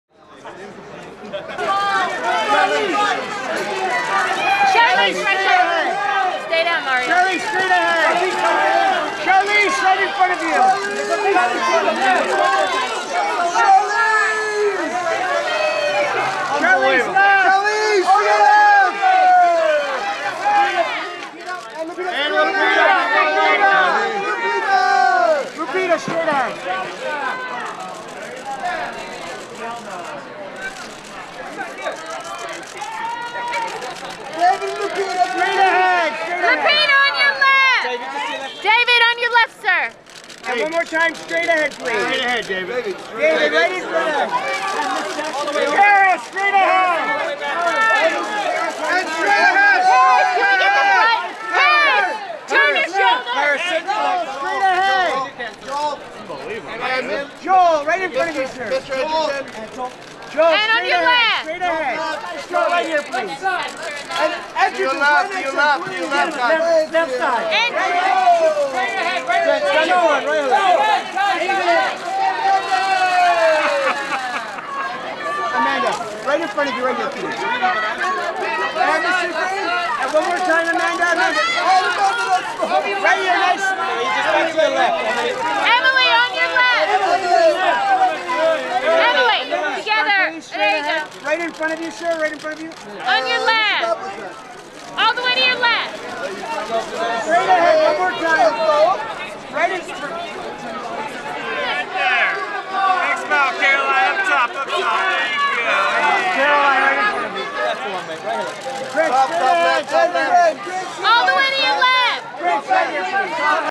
Charlize Theron, Lupita Nyongo & David Oyelowo at the Gringo Premiere
Coverage of the Gringo Premiere with Charlize Theron, Lupita Nyongo, David Oyelowo, Paris Jackson, Joel Edgerton, Nate Edgerton, Amanda Seyfried, Emily Deschanel, Sharlto Copley, Caroline DAmore, & Prince Jackson, on March 6, 2018.